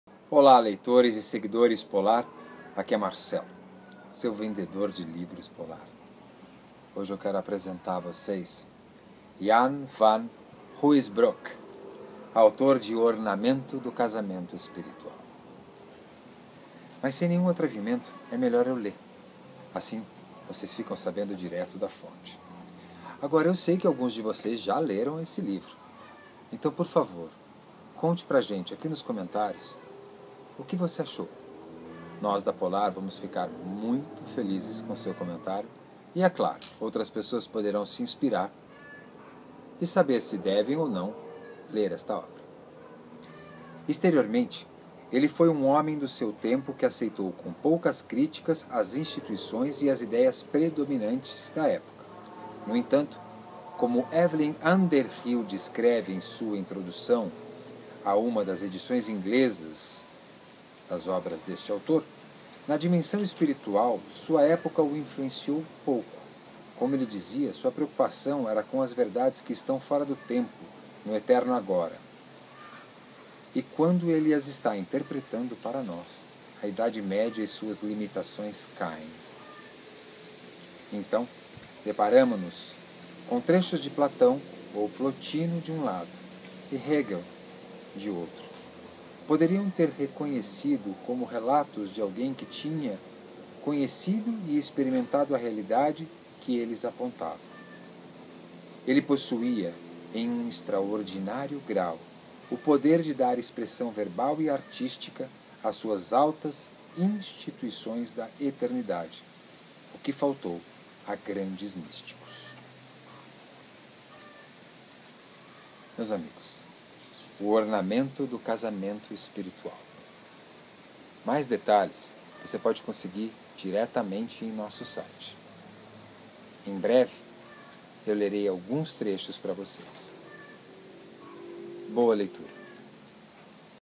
ORNAMENTO DO CASAMENTO ESPIRITUAL - Jan Van Ruysbroeck - Obra clássica do séc. 14
O Ornamento do Casamento Espiritual de Jan van Ruysbroeck é o maior místico flamengo e um dos maiores dentre todos os contemplativos cristãos. Uma de suas obras mais importantes é apresentada aqui pela primeira vez para os leitores da língua portuguesa. Ele nasceu em 1293, no pequeno vilarejo de Ruysbroeck ou Ruusbroeck (próximo de Bruxelas), de onde vem seu nome. Segundo os biógrafos, aos onze anos ele fugiu de casa e foi para Bruxelas, onde teve uma educação religiosa, que incluía uma formação ampla em teologia e em filosofia, áreas do conhecimento para as quais demonstrou enorme aptidão. Como Evelyn Underhill descreve em sua Introdução à uma das edições inglesas de obras do autor, “na dimensão espiritual, sua época o influenciou pouco. Como ele dizia, sua preocupação era com verdades que estão ‘fora do Tempo’, no Eterno Agora; e quando ele as está interpretando para nós, a Idade Média e suas limitações caem. Então, deparamo-nos com trechos que Platão ou Plotino de um lado, e Hegel de outro, poderiam ter reconhecido como relatos de alguém que tinha conhecido e experimentado a Realidade que eles apontavam. (…) ele possuía, em um extraordinário grau, o poder de dar expressão verbal e artística às suas altas intuições da Eternidade, o que faltou a tantos grandes místicos”. Em 1343, quando ele tinha 50 anos, o crescente sentimento do contraste entre essas intuições, o excessivo formalismo religioso e a agitação da cidade alcançou um ponto que se tornou insuportável, de modo que ele partiu de Bruxelas para sempre. Instalou-se, com dois outros colegas sacerdotes, num antigo heremitério, na floresta de Soignes. Com o passar dos anos, seus grandes dons místicos tornaram-se conhecidos e vários discípulos se mudaram para lá, dando origem a uma pequena comunidade. Ruysbroeck faleceu no dia 2 de dezembro de 1381, aos 88 anos de idade.

O “Vendedor da Polar” é o produtor, diretor, ator e amigo @marcelopaganottiator

Livro: O Ornamento do Casamento Espiritual
Autor: Jan van Ruysbroeck